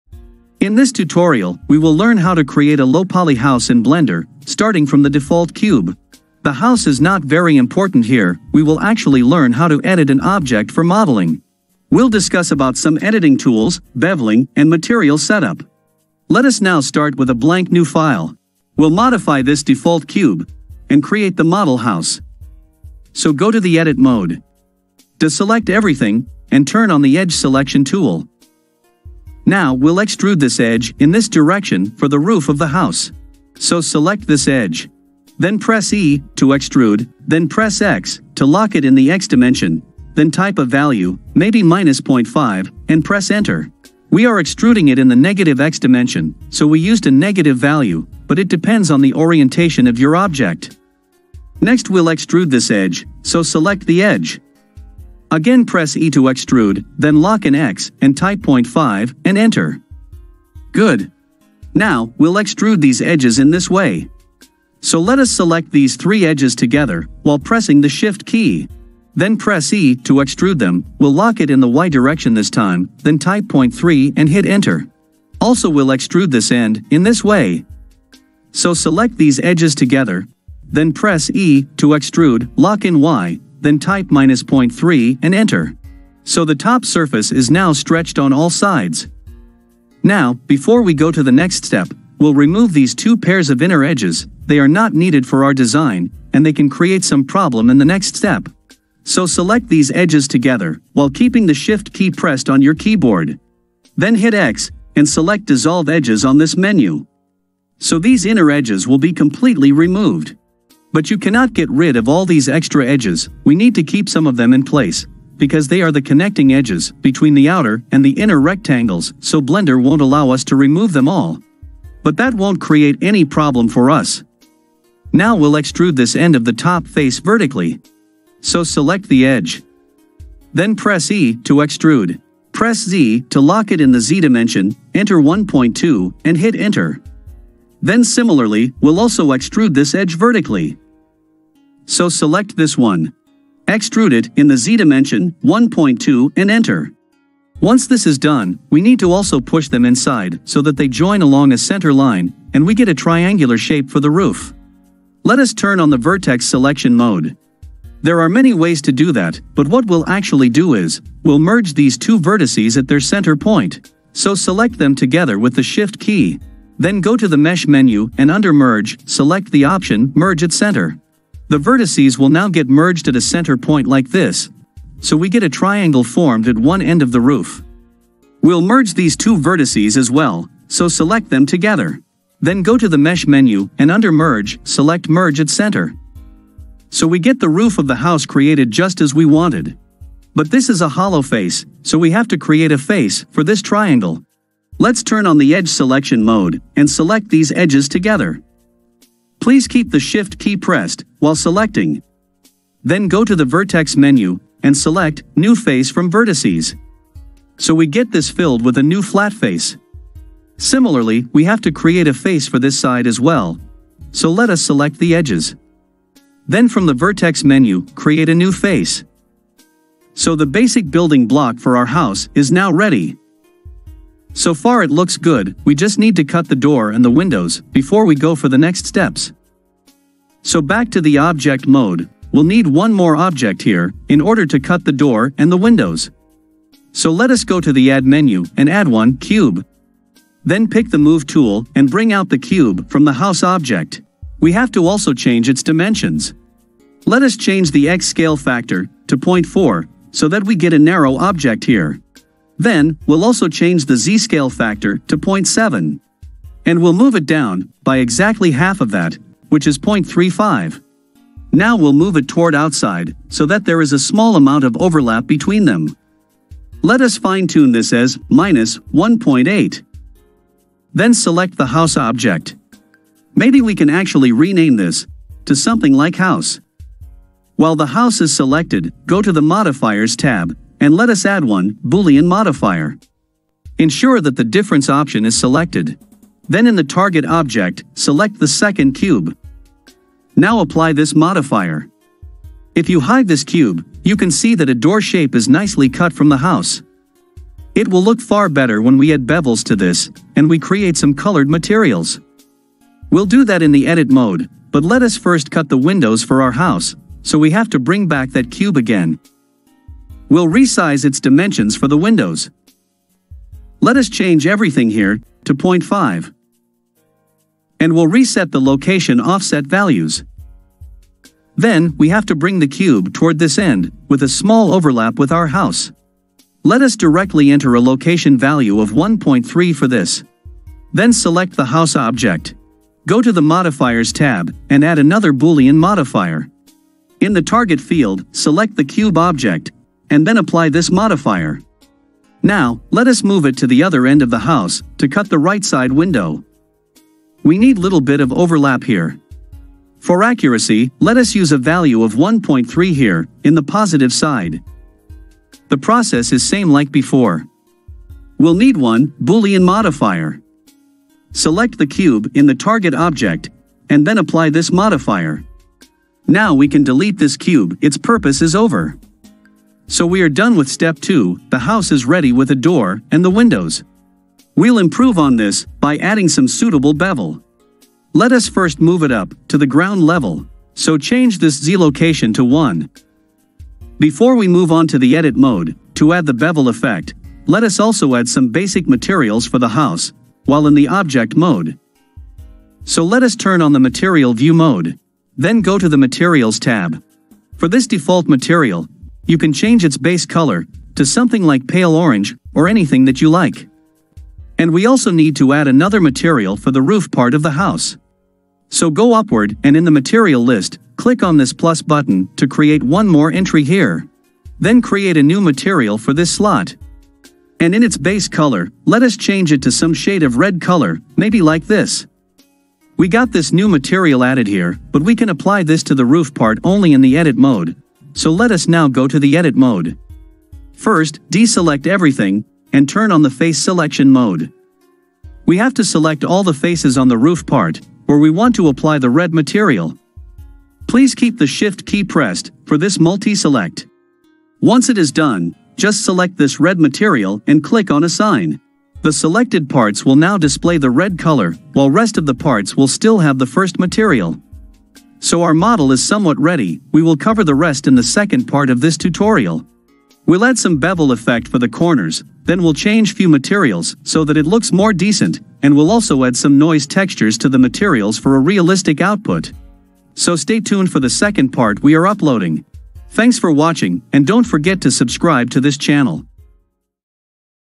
Low Poly House In Blender | Easy Modeling : Part-1 | Mesh Editing - Bevel - Materials
In this tutorial, we have discussed about editing & modeling in Blender. This is primarily for beginners to learn the basic steps in creating any model. We have divided this tutorial into two parts. In this first part, we have discussed how to edit a mesh to create any model, and how to use boolean modifier. In the second part, we will discuss how to add bevels and how to create some suitable materials for your model. Please watch the video for the complete tutorial.

Timestamp
00:00 Start with the default cube
00:25 Edit the cube & model a house
03:50 Create a door & some windows
06:32 Add some basic materials